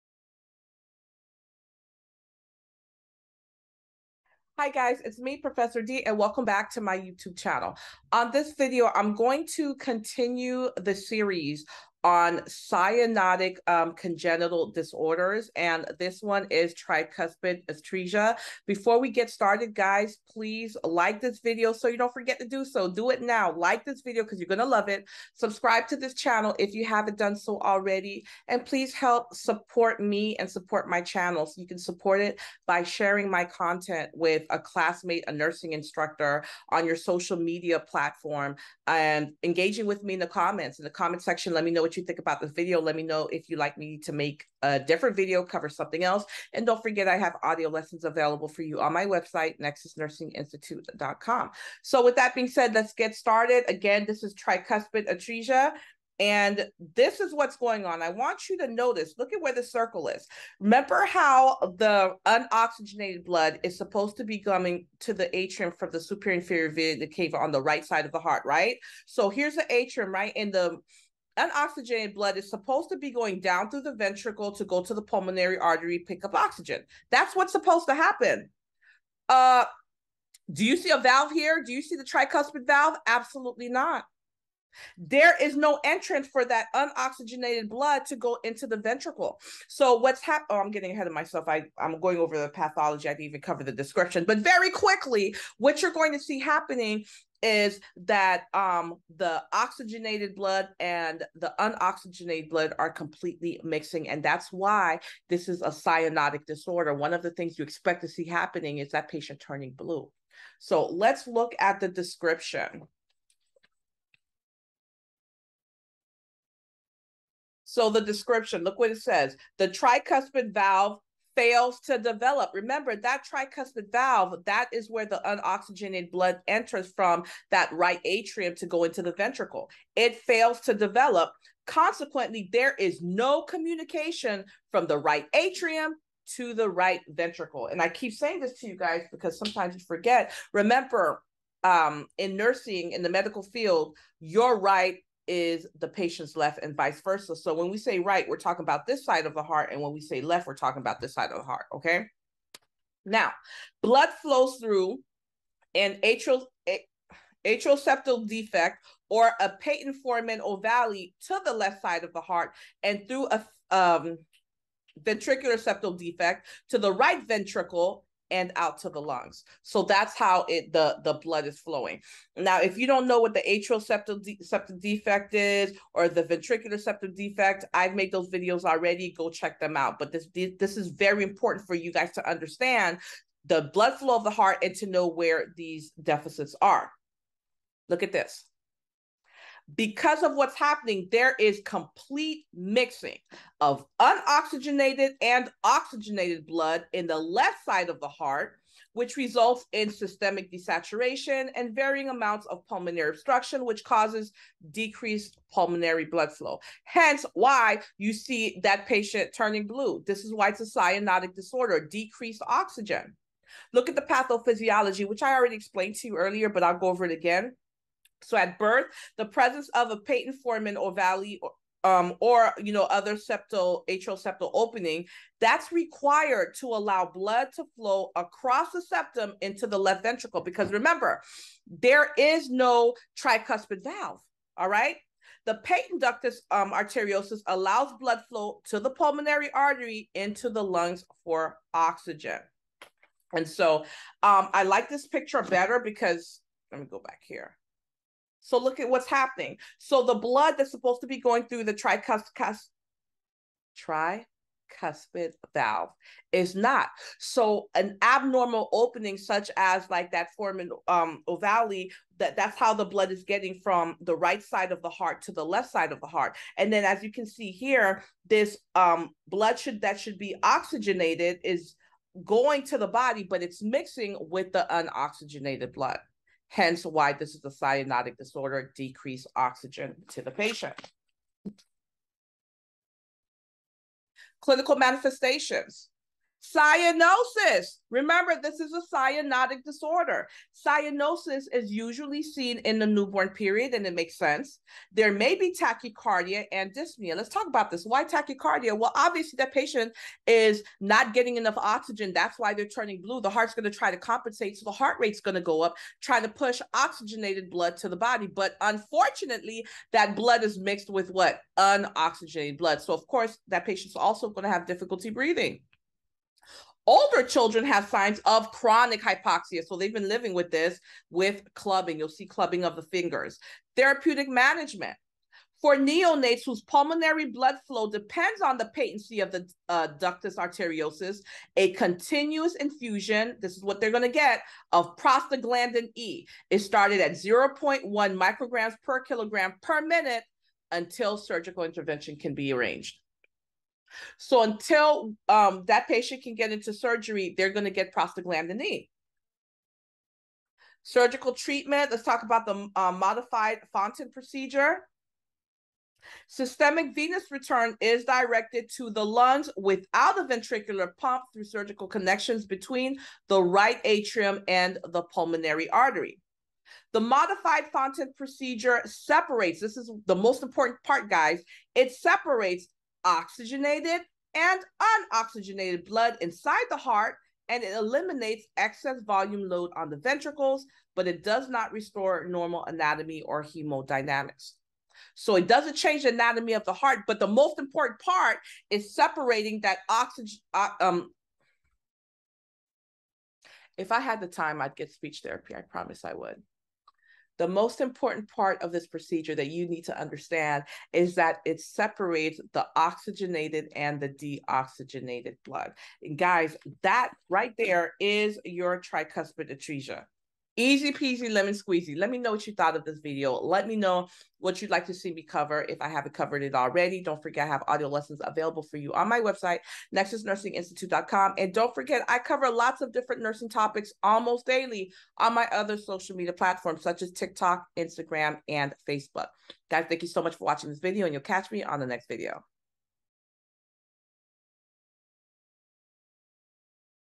Congenital Heart Disorders: Tricuspid Atresia
In this video, you will learn about the differences in congenital heart disorders, the clinical manifestations, and treatment options. This is a great pediatric video to add to your nursing studies.

Follow Nexus Nursing on TikTok, Facebook, and Instagram!!!

Gifts of appreciation welcome!!!